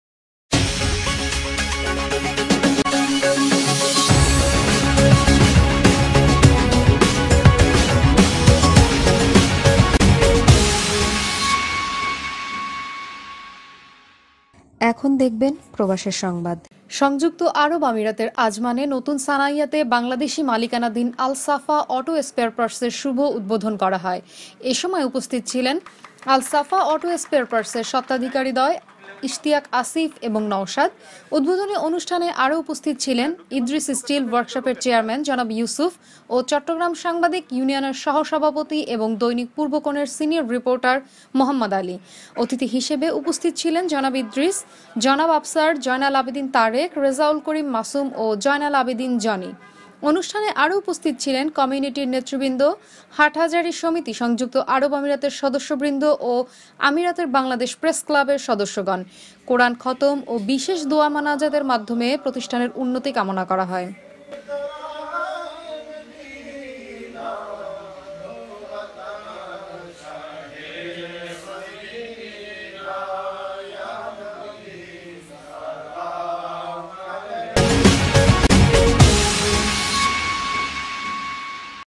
AL SAFA AUTO SPARE PARTS এর শুভ উদ্বোধন
সংযুক্ত আরব আমিরাতের আজমানে  নতুন  ছানাইয়াতে বাংলাদেশী    মালিকানাধীন  AL SAFA AUTO SPARE PARTS এর শুভ উদ্বোধন করা হয়।